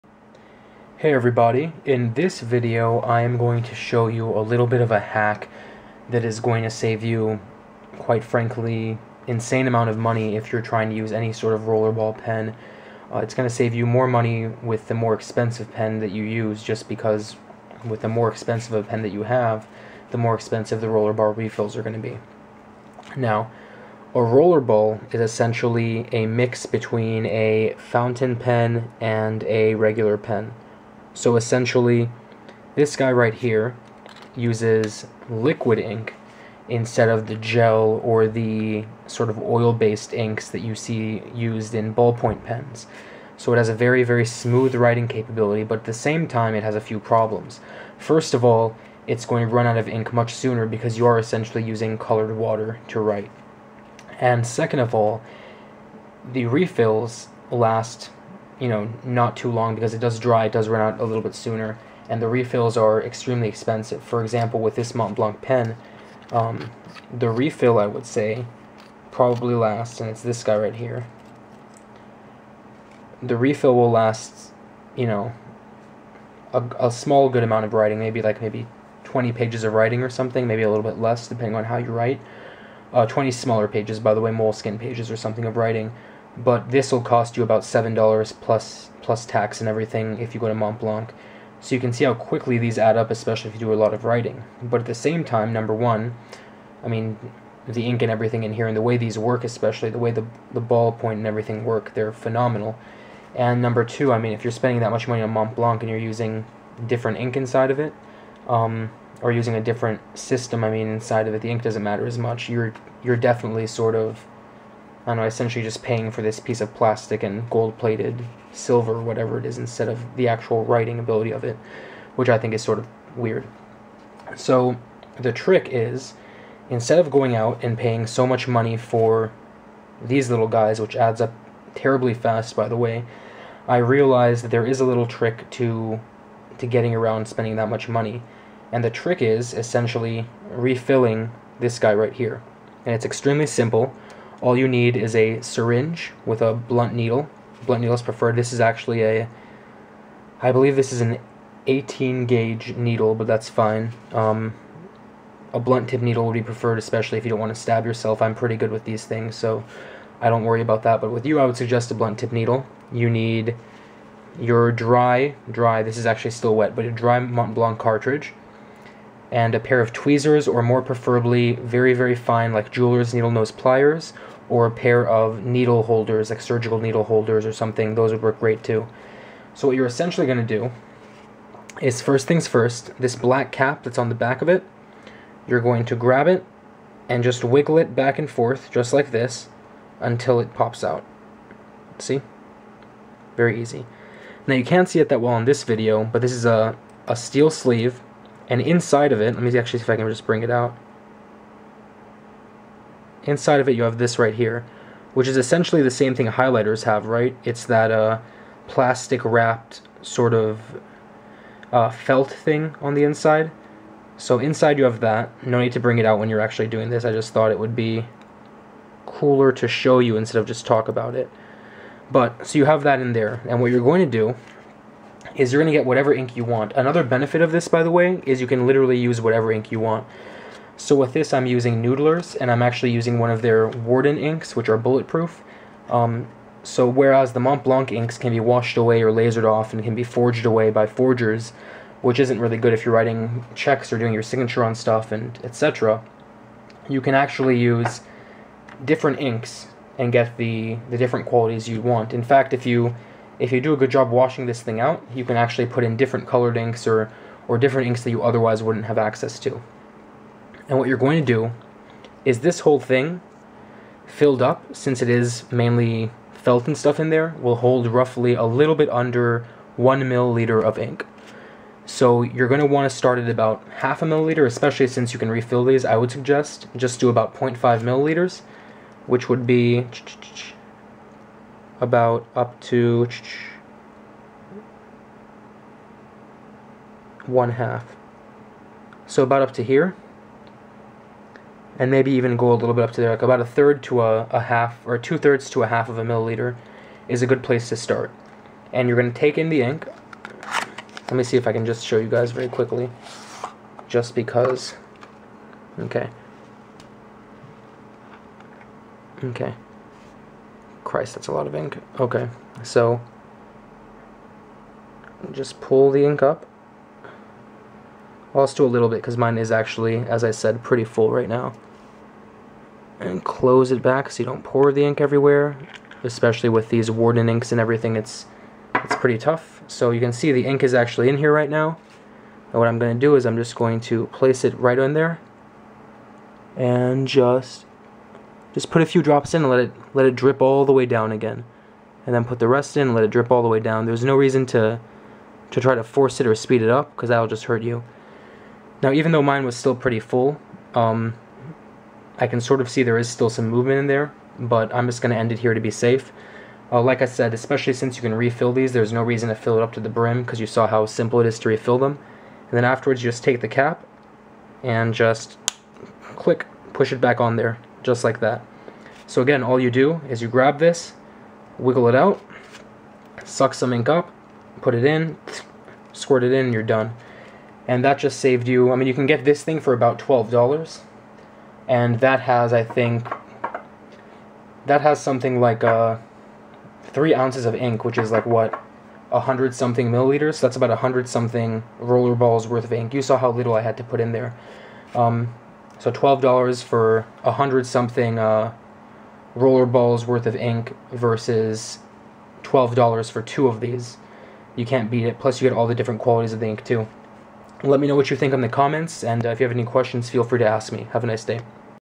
Mont Blanc Rollerball HACK— Save $$$ on Refills (Works on ALL Rollerballs!)
-- Montblanc and Pen Gear at a MAJOR discount!

Learn how to stop paying $7+ per 20 pages with the overpriced Mont Blanc rollerball refills... Instead, pay ~$12/100 refills. AND still use the REAL Mont Blanc rollerball!

Montblanc Rollerball
Montblanc Rollerball Hack
Rollerball Hack
Rollerball Refill Hack
Montblanc Rollerball Refill Hack